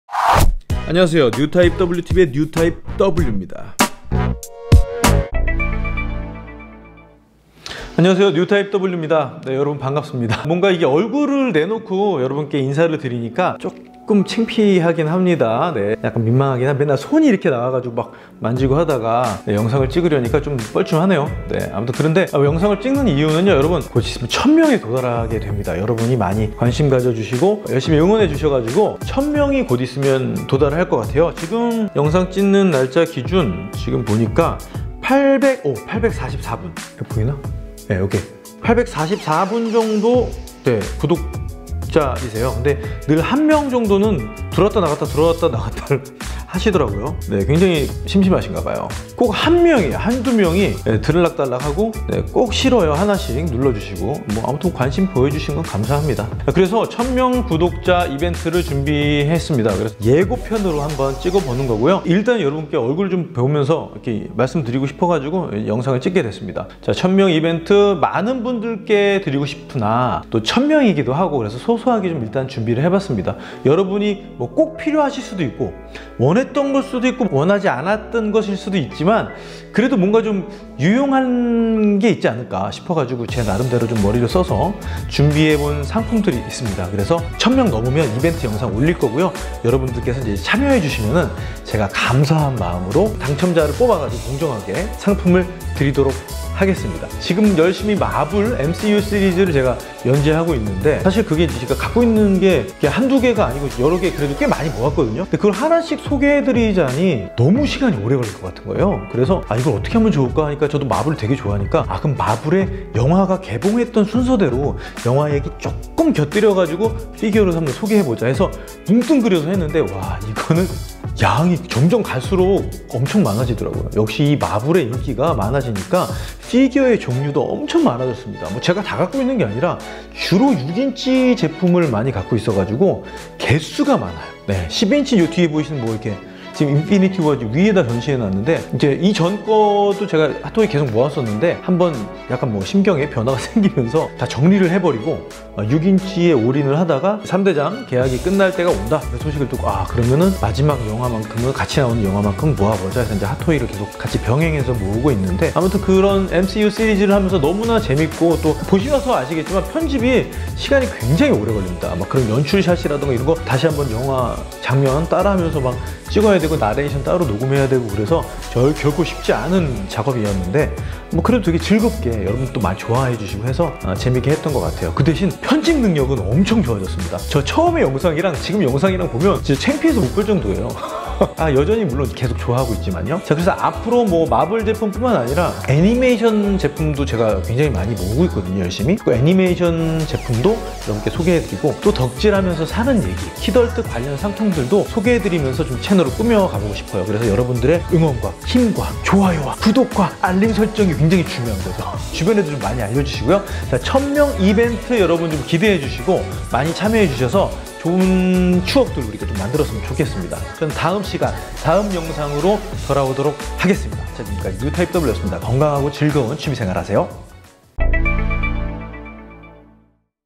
뉴타입W TV 사랑해주시는 구독자 분들께 천명이 넘으면 선물을 드리려고 합니다!!
아직 조금 남아있지만 천명이 얼마 남지 않아  미리 이벤트를 공지하려고 합니다!!

큰 것은 아니지만 소소하지만 유용한 것으로 준비해 봤습니다~

이벤트 예고와 함께 채널 얘기도 살짝 했어요.

많은 분들이 함께 해주시면 좋겠습니다!!^^

영상을 끝까지 봐주시고 응원해주시는 여러분~

감사하고 사랑해요~~♡♡♡